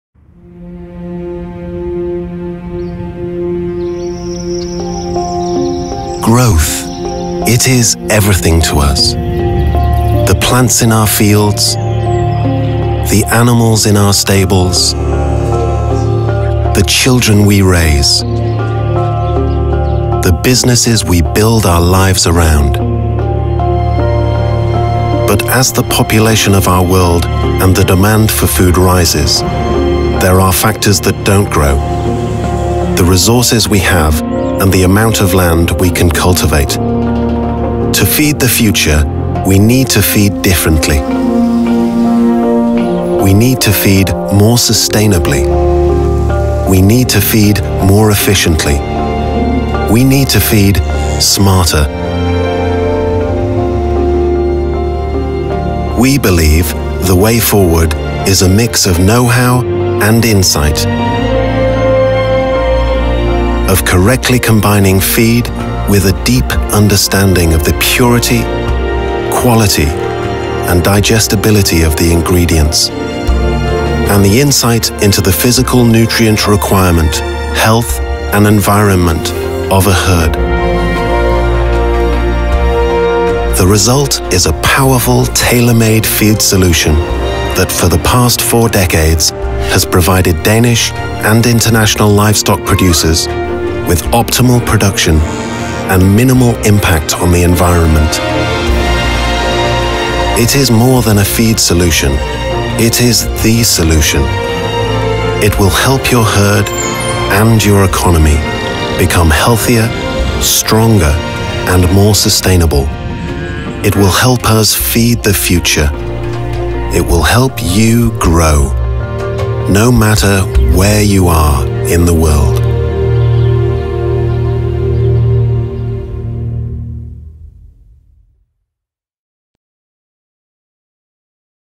Vilomix Group Corporate Movie
Customised vitamin and mineral solutions combined with feed additives in sustainable feed solutions is our specialty - always with the aim to ensure the best and most economical results in your animal production.

Watch our corporate movie here on what we do.